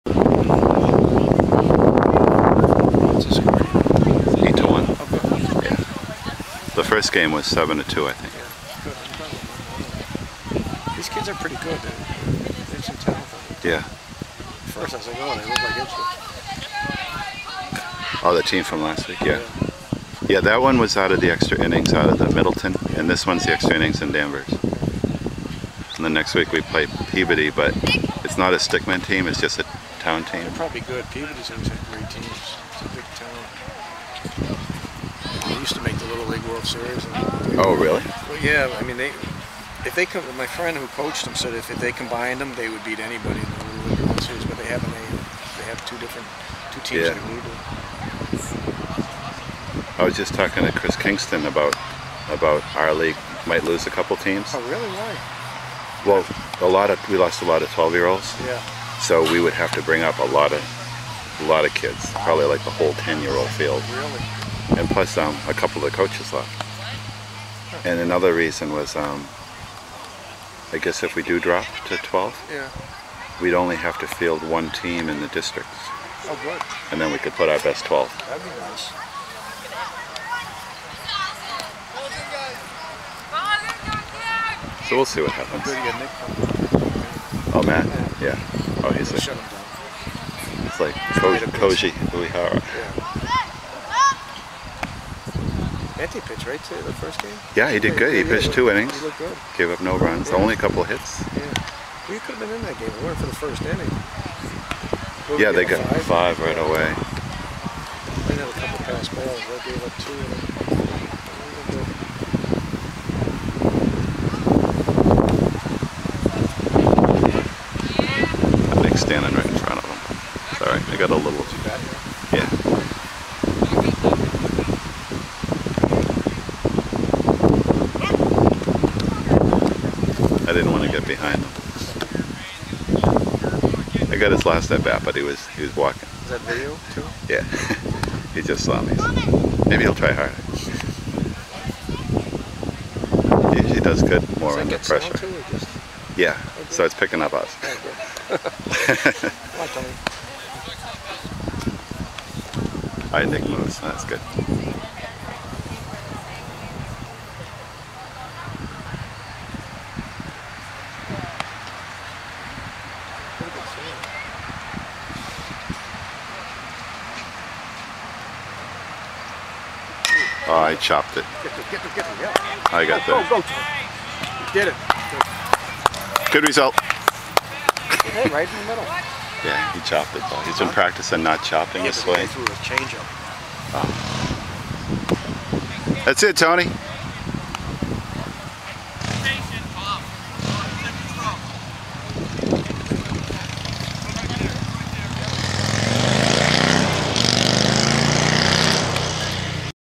Choppy Choppy (.avi)
Tony playing fall ball with the Owlz, chopping a base hit up the middle...